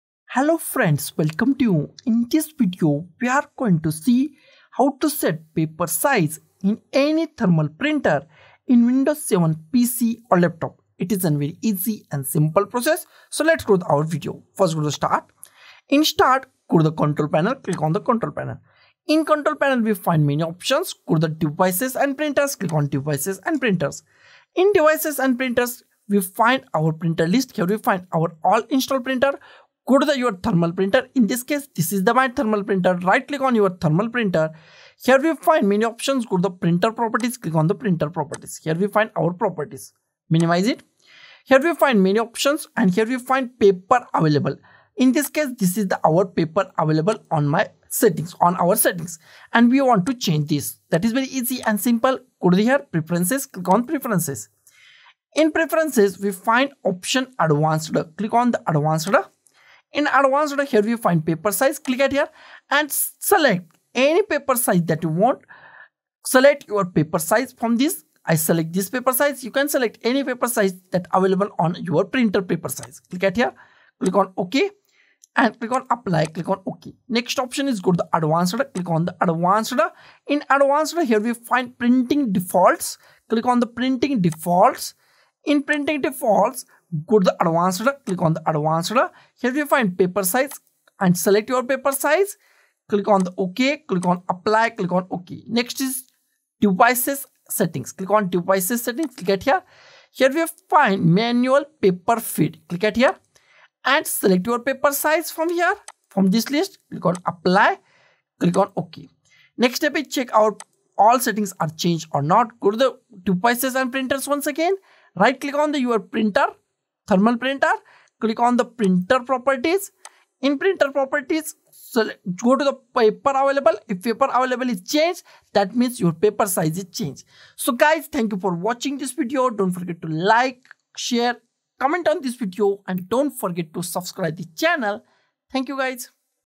How to Set Paper Size in Any Thermal Printer in Windows 7 PC or Laptop
In this video, I’ll guide you through the process of setting the paper size in any thermal printer on a Windows 7 PC or laptop. Whether you're printing labels, receipts, or custom-sized documents, this step-by-step tutorial will show you how to adjust the paper size settings for your thermal printer to ensure accurate printing results.

📌 Topics Covered:
How to access printer settings in Windows 7
Setting custom paper sizes in thermal printers
Troubleshooting paper size issues in Windows 7